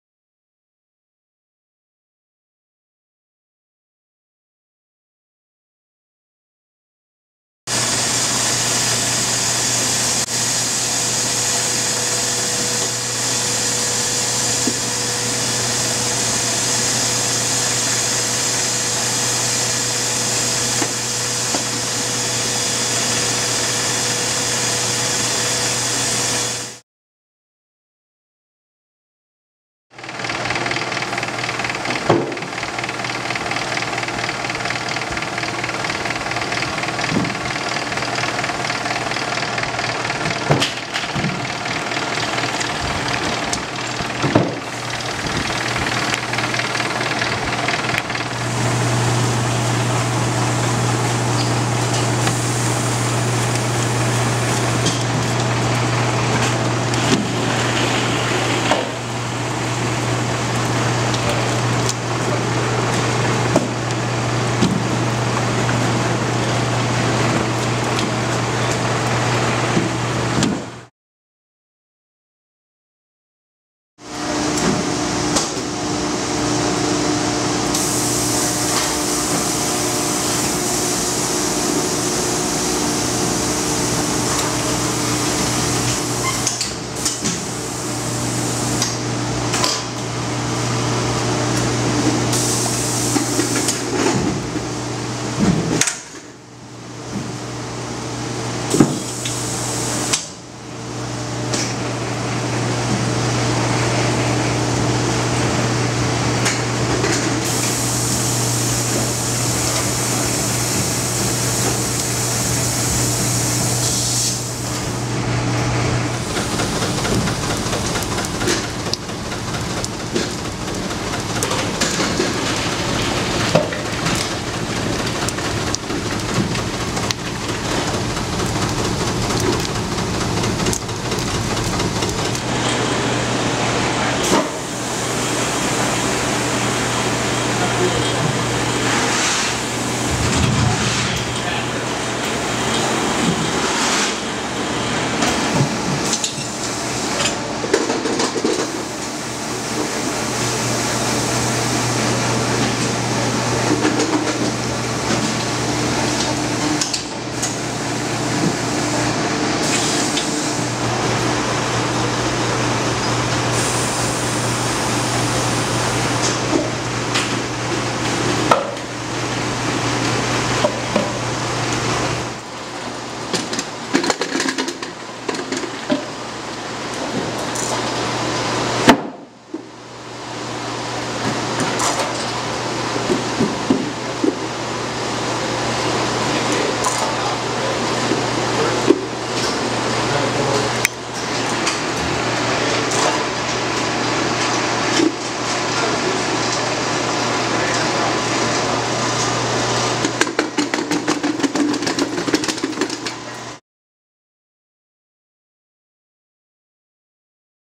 Extracting Honey
How we extract our honey here at Desert Creek Honey with our 120 Frame Air Ram Cowen Extractor!